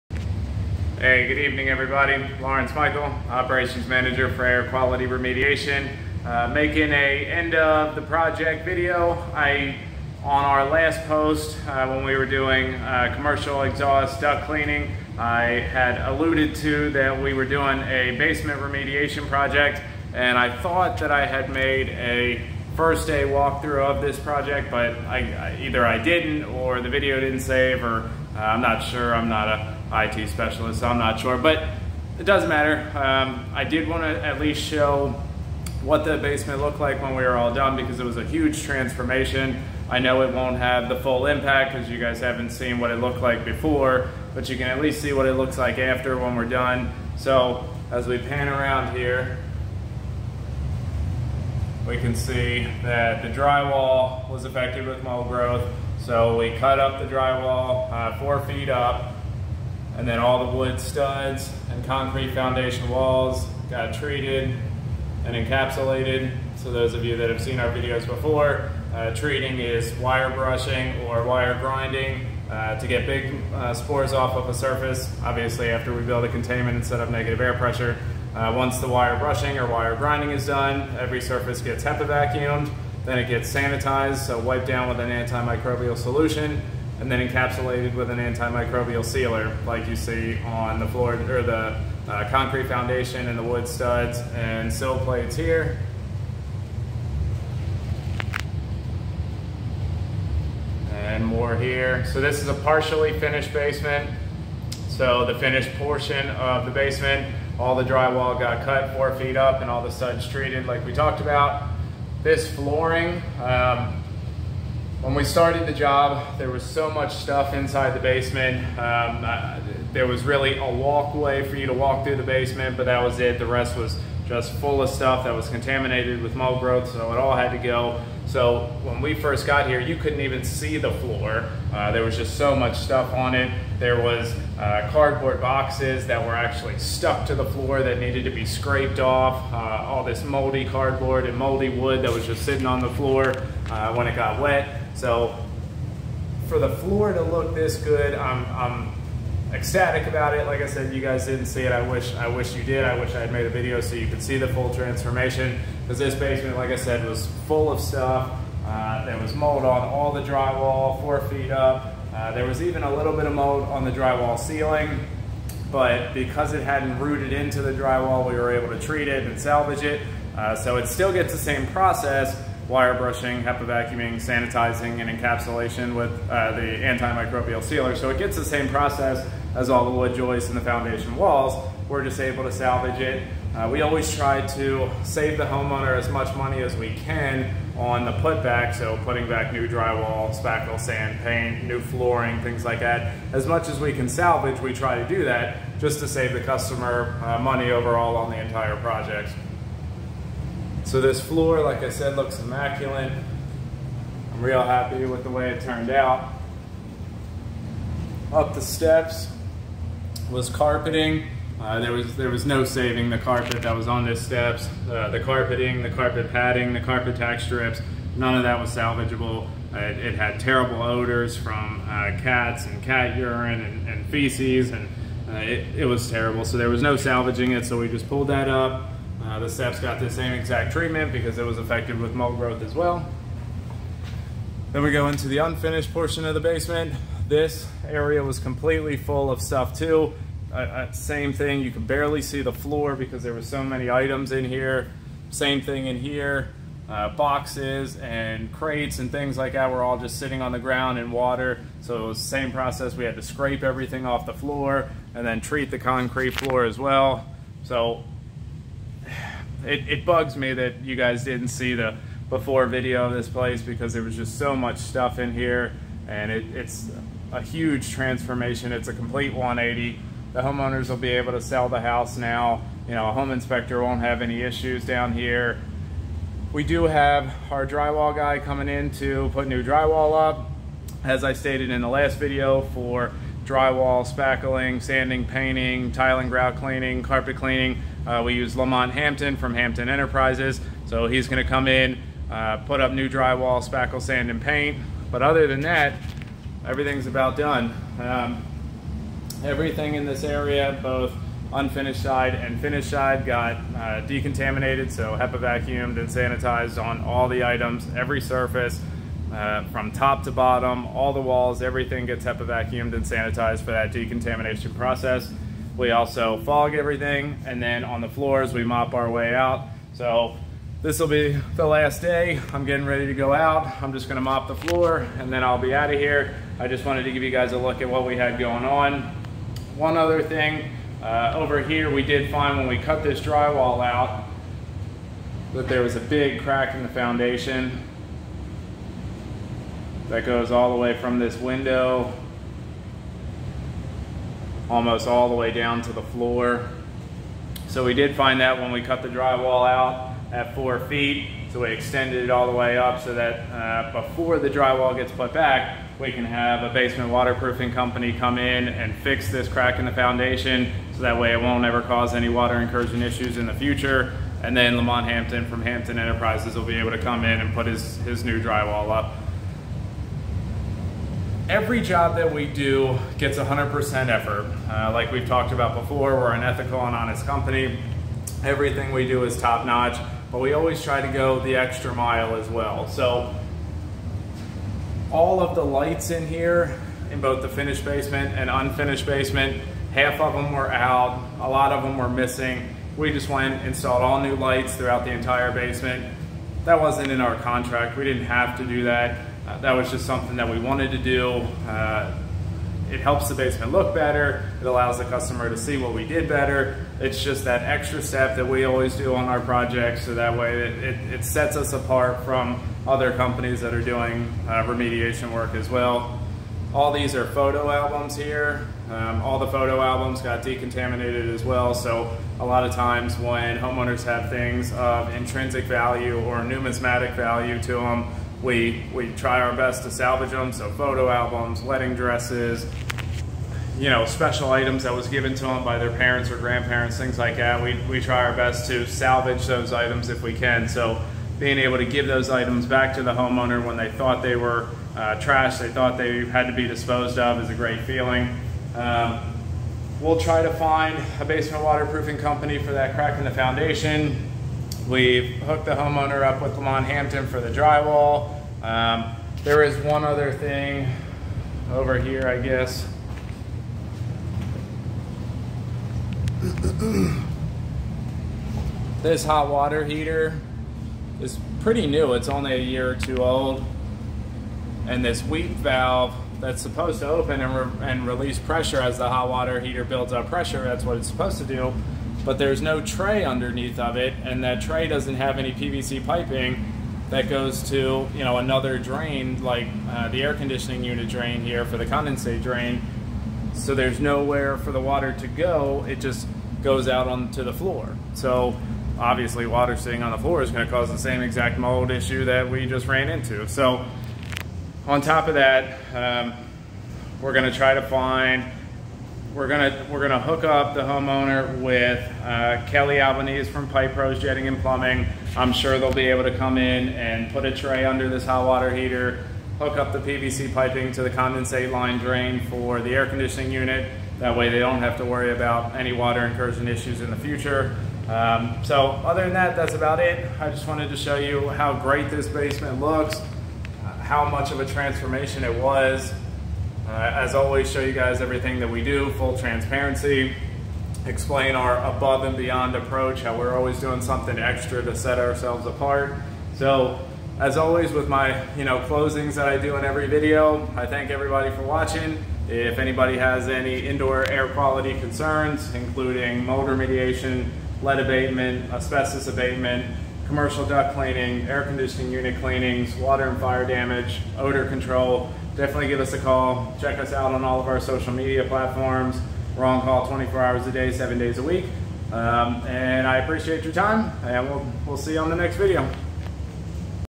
Basement Mold Remediation Project HUGE TRANSFORMATION!!!!
hey everyone here is a look at the completed mold remediation project in a basement we have been working on for about 8 days. this basement is now safe and looks and smells great. now our drywall and painting guy will come in to finish everything up and the house can be sold. if anyone has any indoor air quality concerns give us a call anytime at . thanks for watching and we'll see you on the next video.